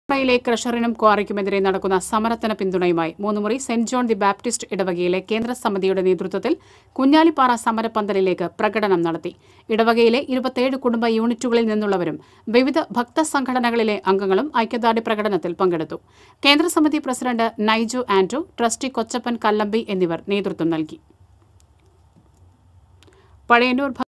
Moonnumury
കുഞ്ഞാലിപാറ സംരക്ഷിക്കണമെന്നാവശ്യപ്പെട്ട്
മൂന്നുമുറി ഇടവക കേന്ദ്രസമിതിയുടെ നേതൃത്വത്തില്‍
 പ്രകടനം